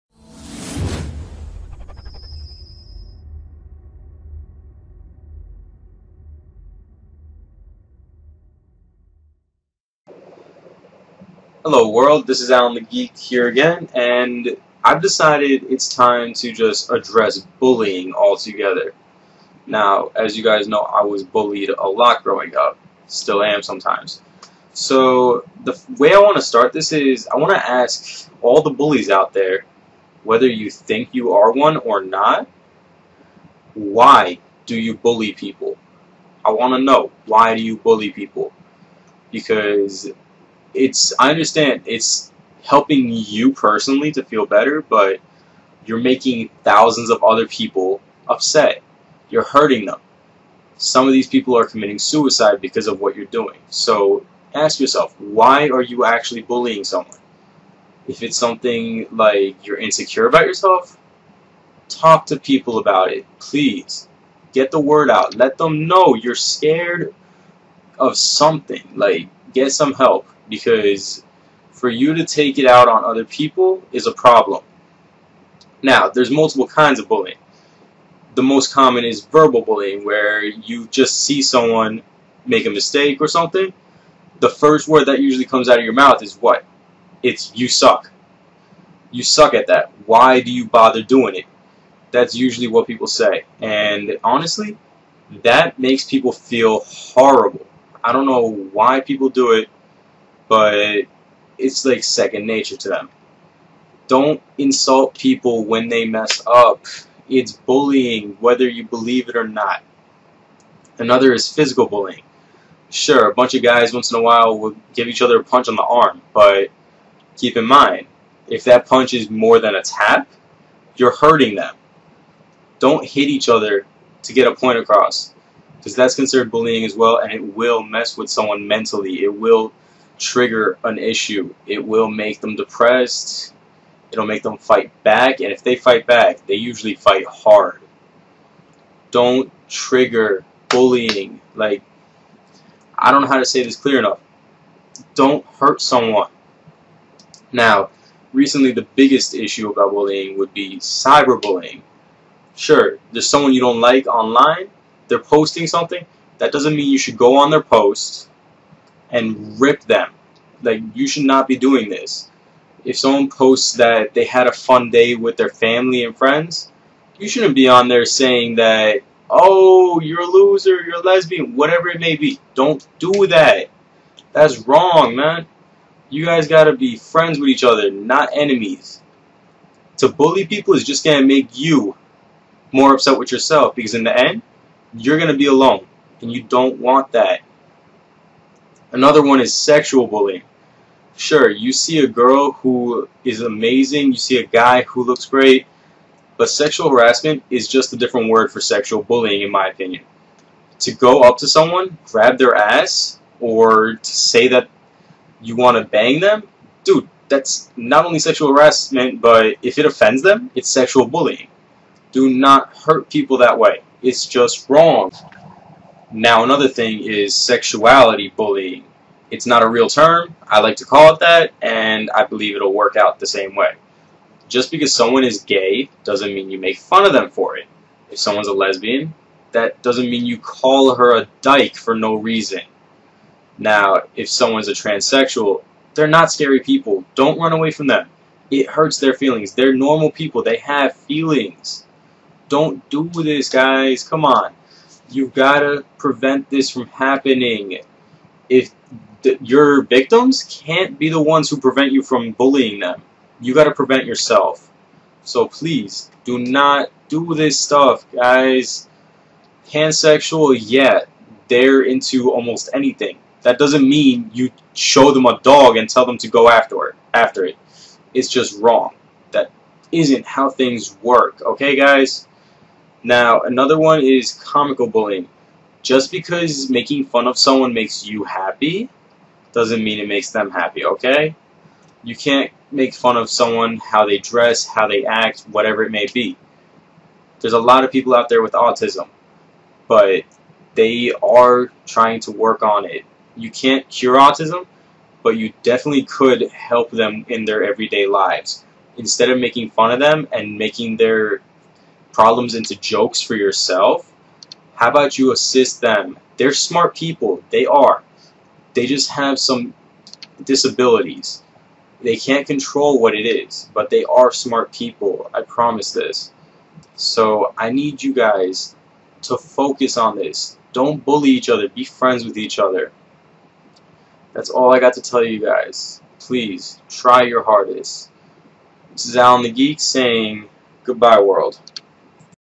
Are You a Bully?: Leave me alone
Are you a bully and you don't even know it?
Why do you bully people?
Let's finally put an end to bullying.